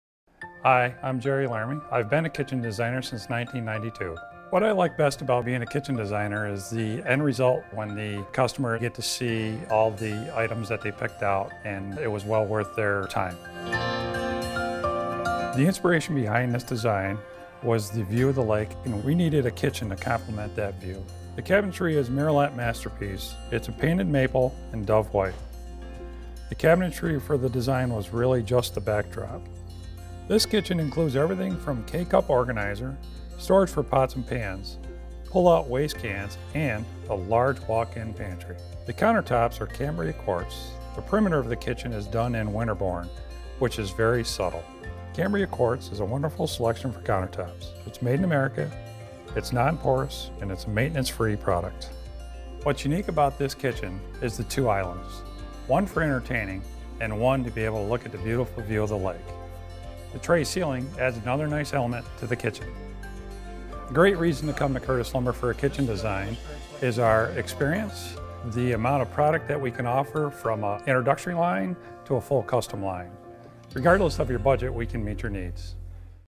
Lake House Kitchen with Tray Ceiling
The inspiration behind the design is the spectacular view of the lake and this kitchen really compliments the view. The homeowners wanted an oversize island, perfect for entertaining family and friends. The second island serves as a breakfast spot where they can enjoy the view. The kitchen is equipped with accessories including: a K-Cup ® organizer to a pull-out spice rack, storage for pots and pans, roll out trash cans and a large walk-in pantry.

The cabinetry is Merillat Masterpiece, painted maple in dove white. The Countertops are Cambria Quartz Winterbourne™ (perimeter) and Montgomery™ (island).